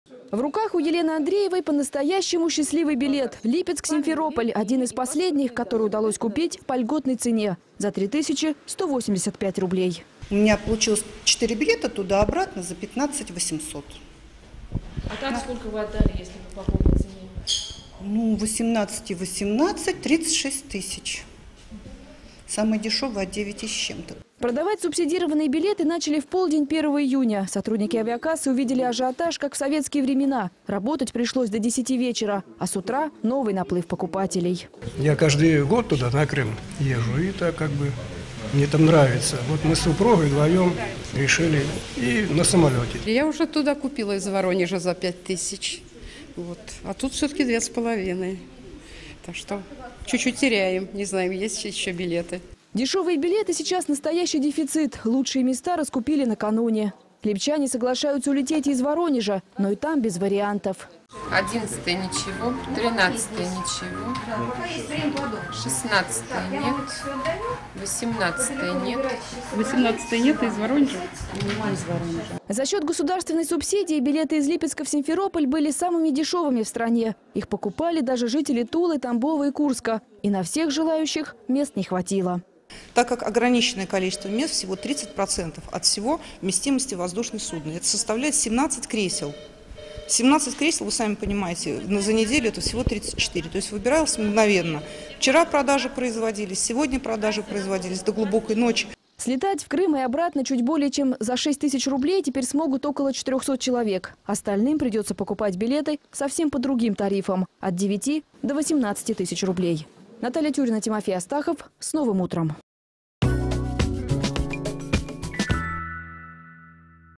Дешевых билетов в Крым больше нет
Дешевых билетов в Крым больше нет. Их раскупили буквально за сутки.    Кассы липецкого аэропорта работали  до трех часов ночи. Такого тут не было с советских времен. Цена в 2 с половиной тысяч рублей привлекала даже жителей соседних регионов. Наталья Тюрина увидела как купили последний льготный билет на рейс Липецк- Симферополь.